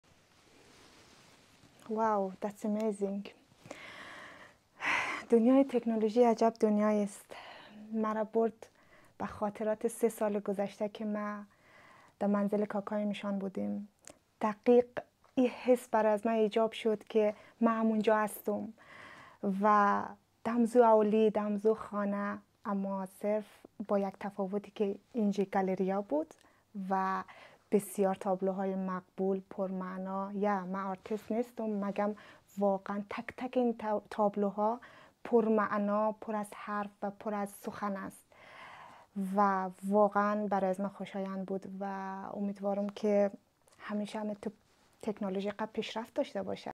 Sharing the VR experience with art lovers - Namad Gallery
Namad Gallery draws its name from the Persian word 'Namad,' meaning felt, a material deeply woven into Afghanistan's cultural heritage.  Founded by Marzia Panahi in 2020, Namad began its journey, envisioning a fusion of Afghan artistry with the contemporary world, and using Namad as the canvas for paintings.

Mina Hosseini - Social activist

“Through these goggles, I felt that I was in my uncle’s house. I am not an artist. However, I know that there lies plenty of stories behind each piece of these paintings and I’m happy that I could visit Namad Gallery virtually.”